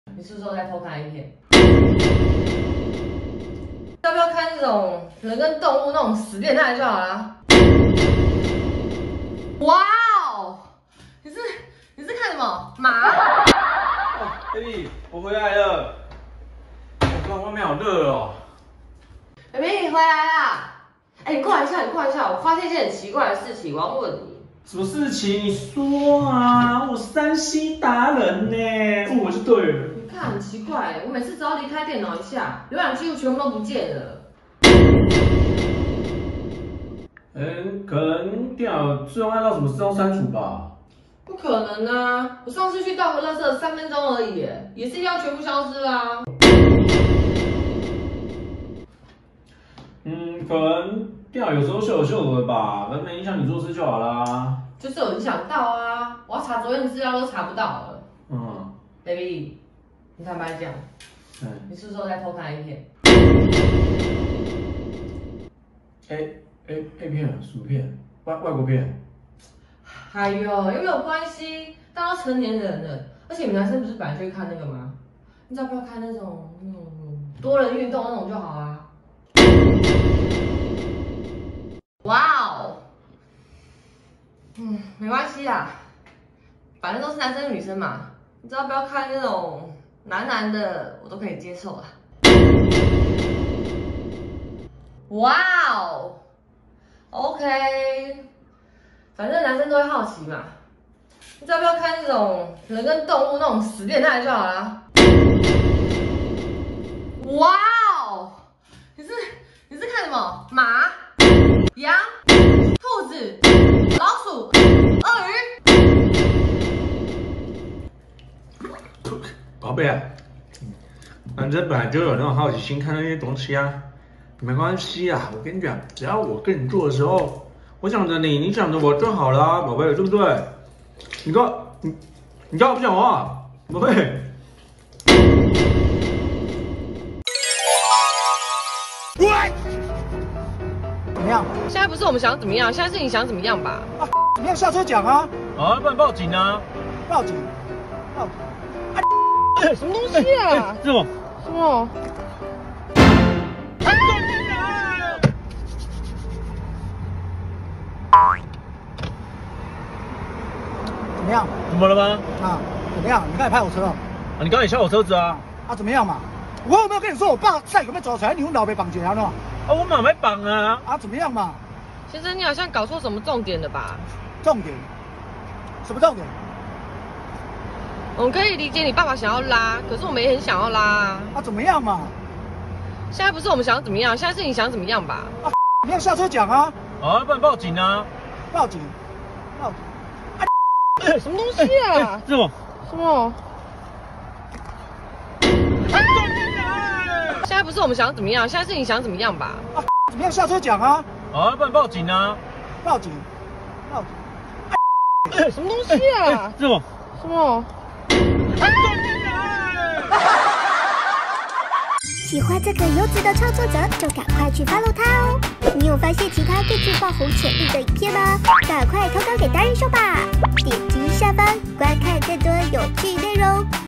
【男友偷看x片被女友抓包？被質問大噴「汗」結局反轉】
笑死🤣直接大噴汗~~(泰泰編)